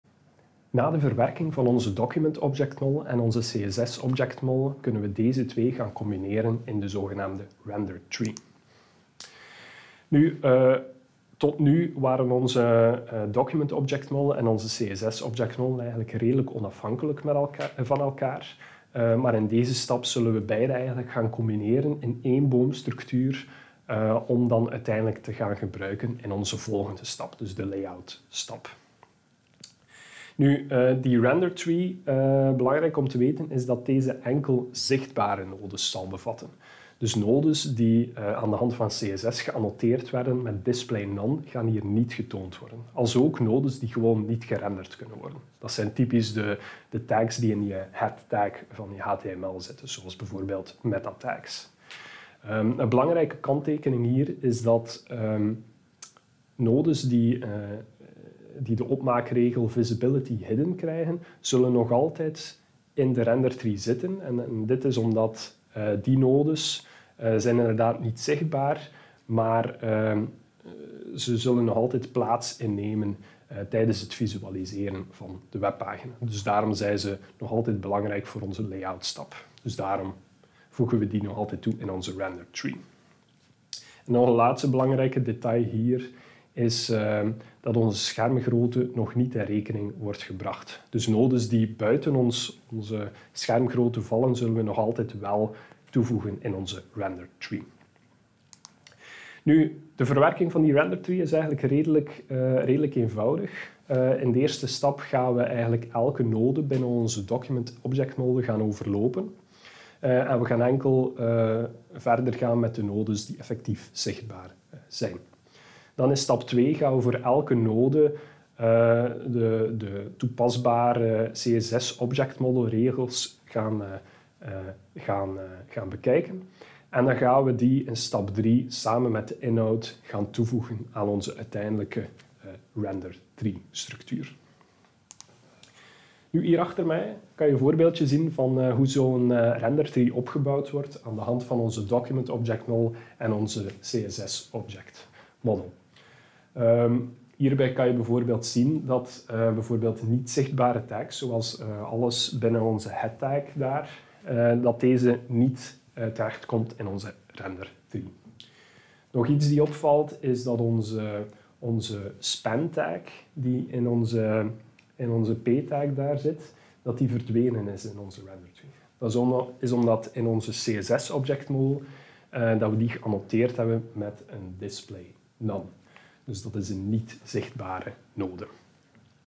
5.4 Web Browsers - Critical Rendering Path - Render Tree
The DOM and CSSOM are combined into the Render Tree.

Speaker: Ruben Taelman

This is part of a video series lecture on Web Browsers.
Part of the Web Fundamentals module of the Ghent University course Web Development.